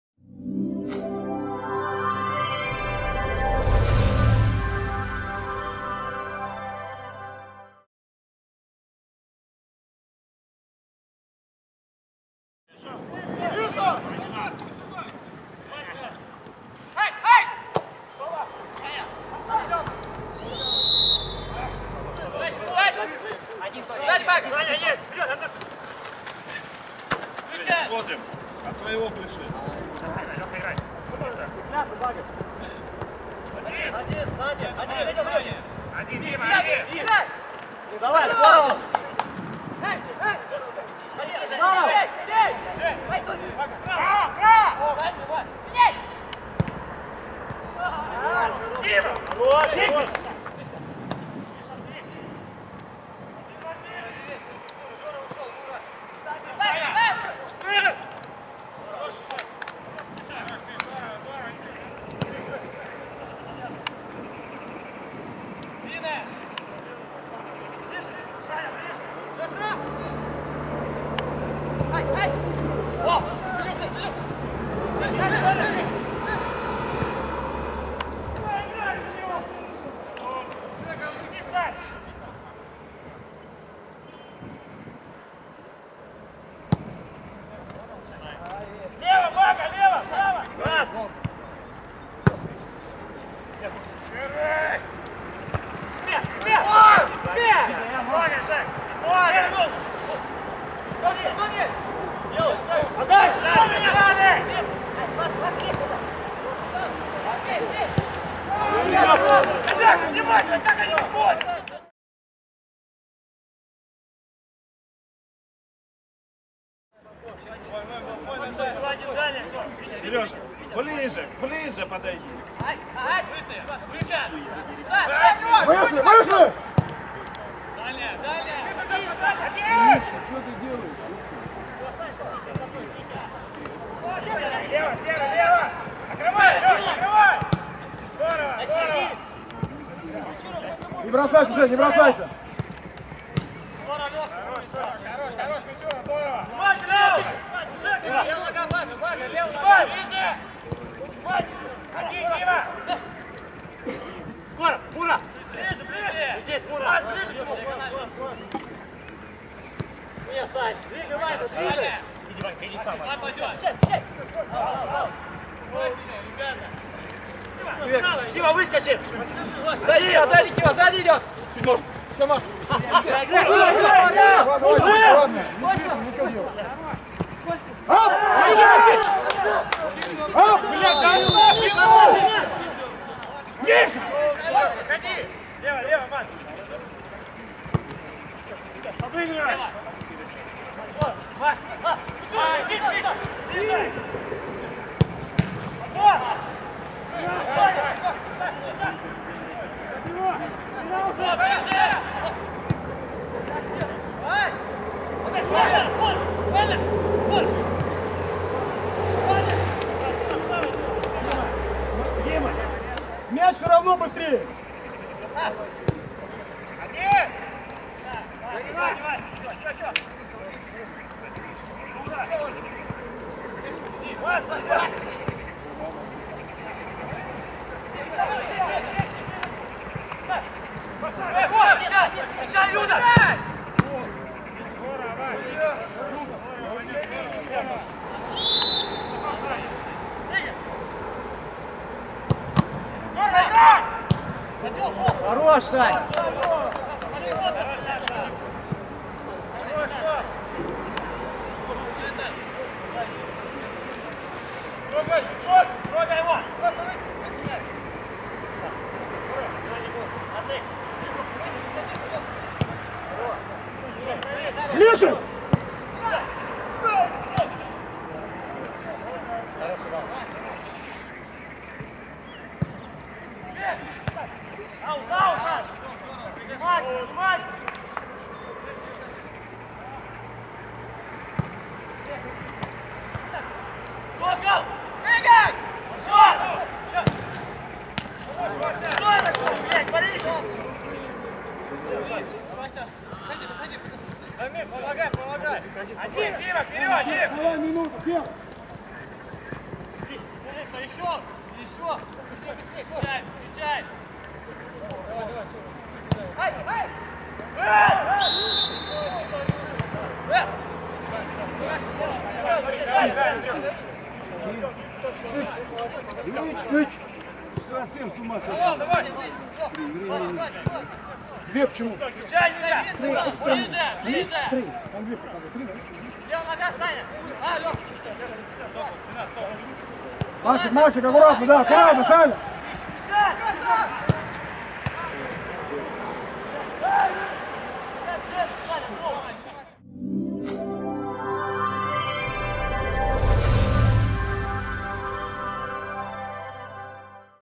22.03.2015_Видеоотчёт о матче Факел-Иртыш(Омск)
22 марта «Факел» провёл первую контрольную игру в рамках стартовавшего тремя днями ранее заключительного тренировочного сбора. В турецком Белеке подопечные Павла Гусева встречались со второй командой зоны «Восток» второго футбольного дивизиона России «Иртыш» из Омска.